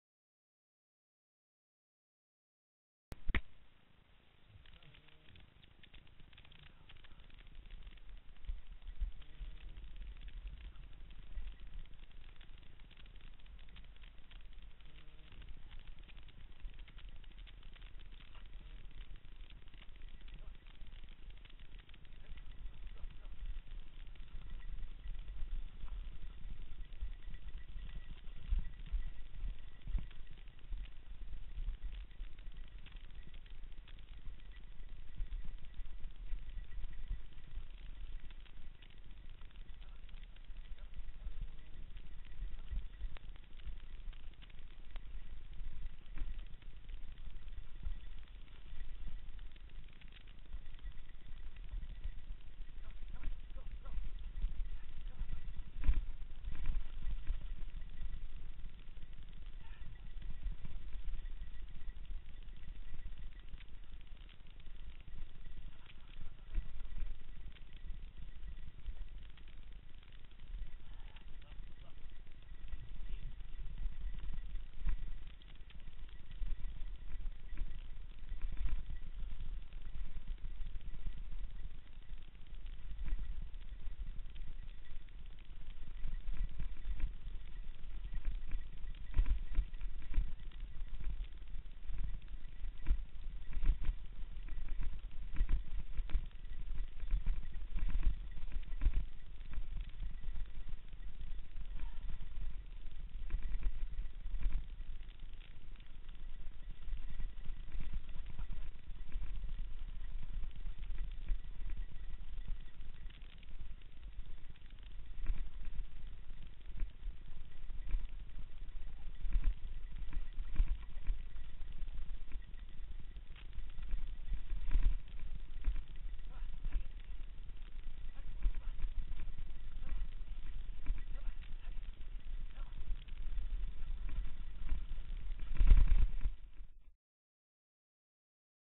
Footbiking Dog scootering on a Diggler .wmv
Bob and Sam footbiking/dogscootering downhill at Kittatinny Valley State Park, Andover, New Jersey